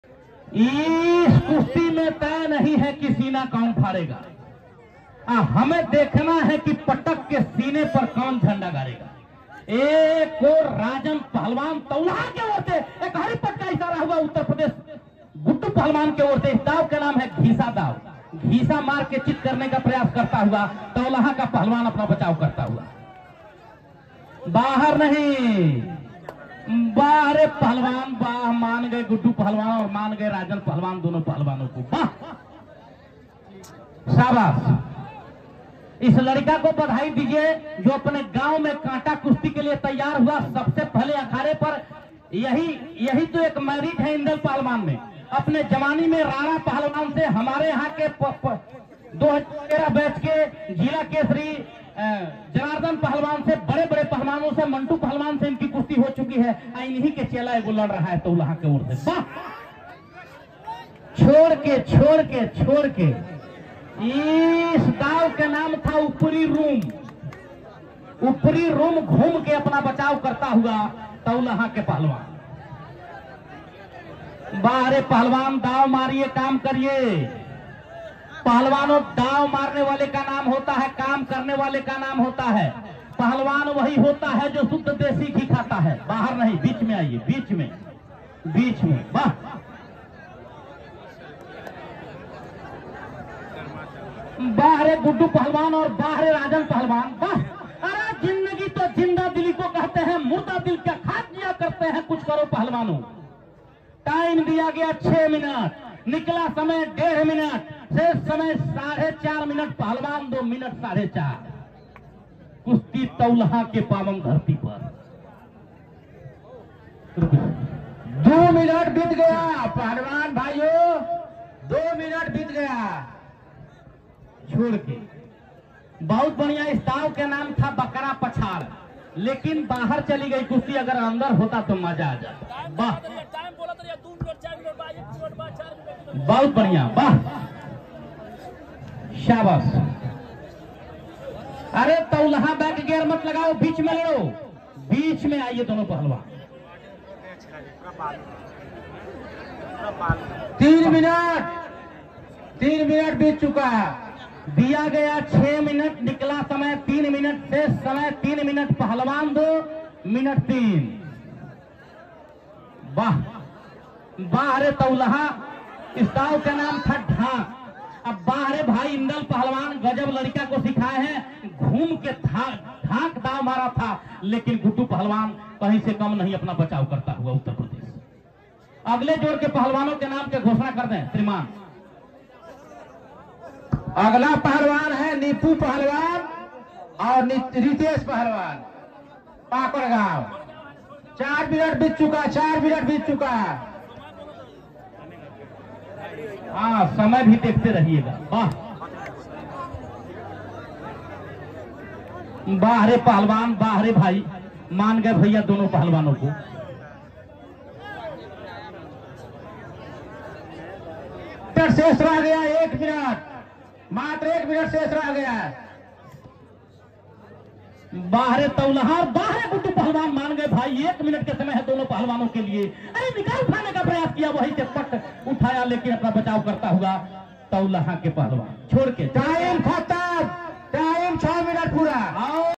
🤼 राजन पहलवान का कुश्ती (तौलाहा विराट दंगल कुश्ती प्रतियोगिता ) प.चम्पारण बिहार
(तौलाहा विराट दंगल कुश्ती प्रतियोगिता ) प.चम्पारण बिहार